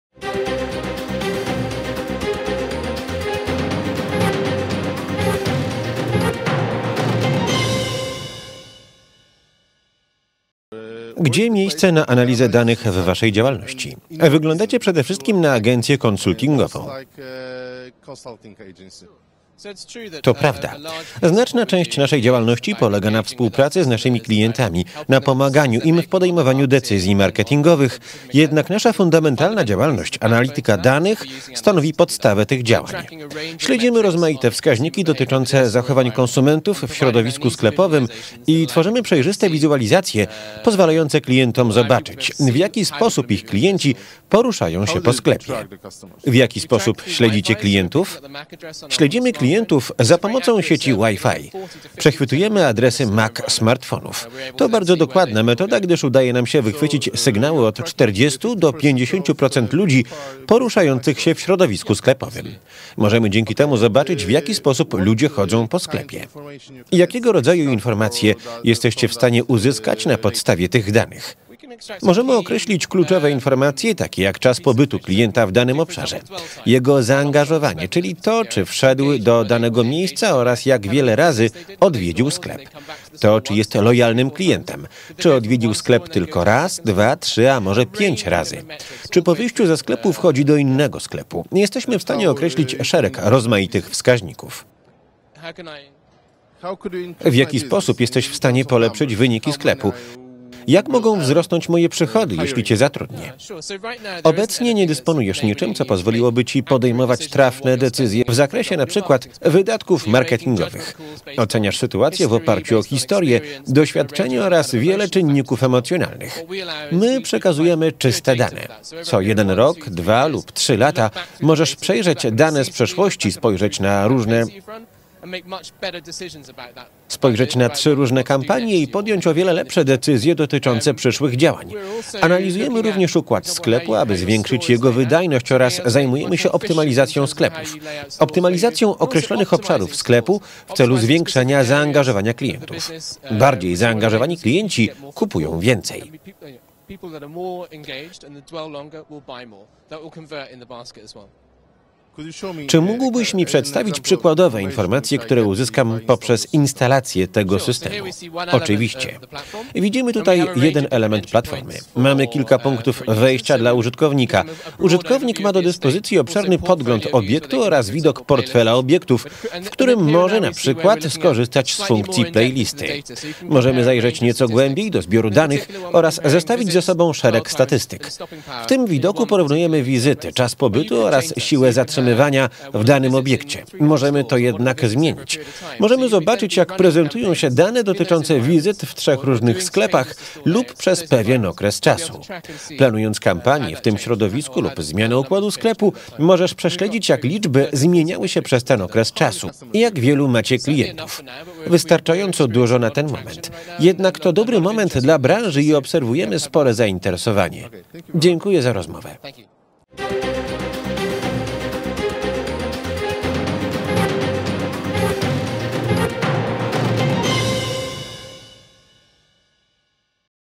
Smartfon zdradza preferencje zakupowe właściciela - Carl Park z Premium Clients
Jednym ze startupów prezentujących się na Targach CeBIT 2014 była firma Premium Clients, która stworzyła aplikację przechwytującą adresy MAC smartfonów, które rejestrują się w danej sieci WI FI. Narzędzie to jest wykorzystywane w sklepach i pomaga analizować zachowanie wielu klientów równocześnie np. odnośnie czasu spędzonego w danym dziale sklepowym. Carl Park, szef startupu opowiedział w wywiadzie dla web.gov.pl, jakie informacje są zbierane w ten sposób oraz jakie wnioski można wyciągnąć na ich podstawie.